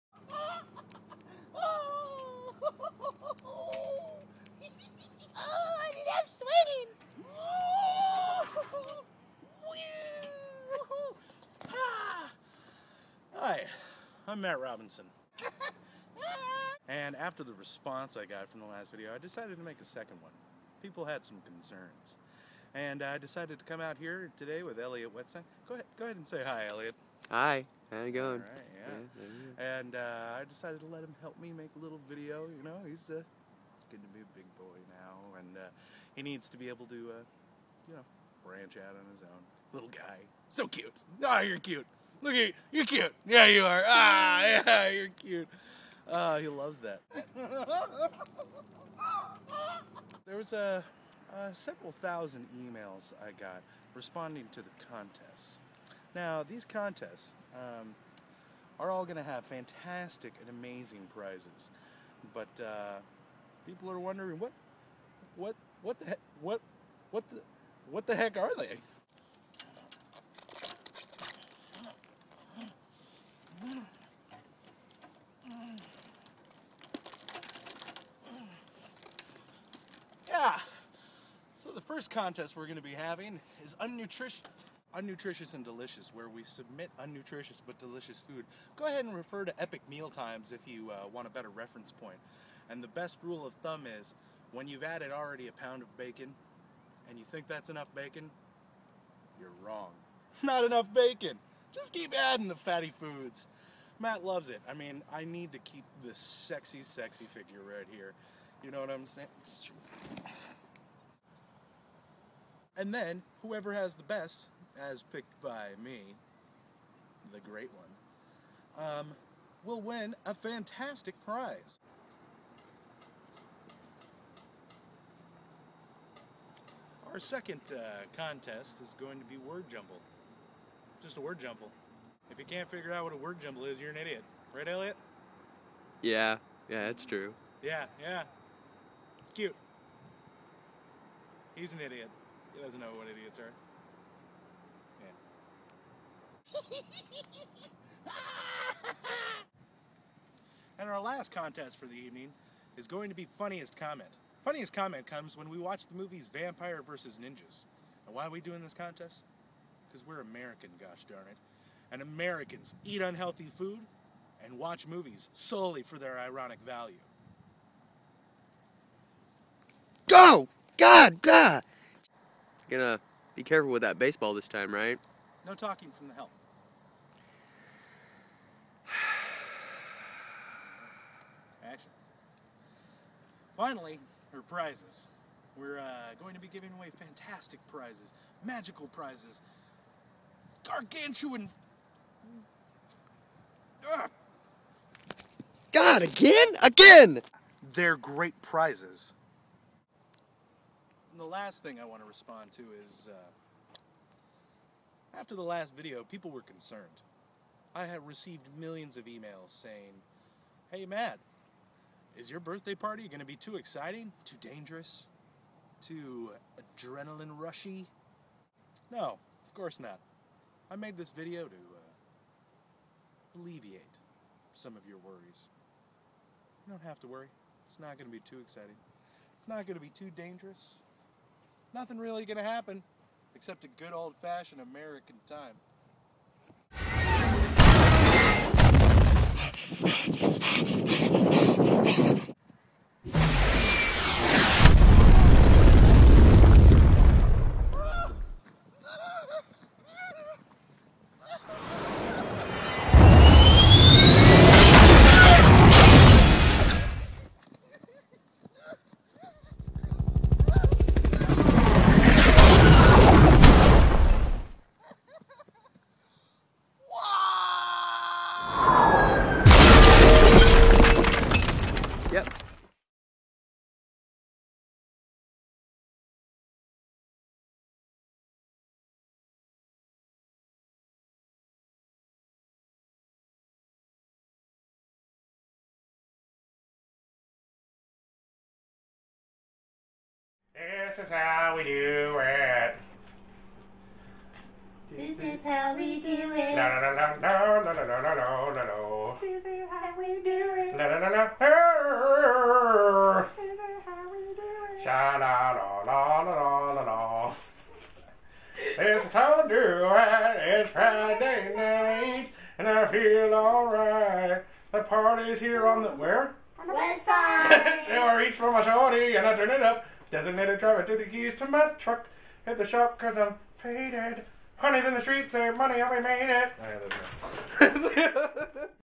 Matt Con Promo 2012 Promo # 2
I decided to make a response video to the millions of e-mails, tweets, & Ham radio operators that had questions about the upcoming Matt Con.

Once again you are welcome for getting to see me.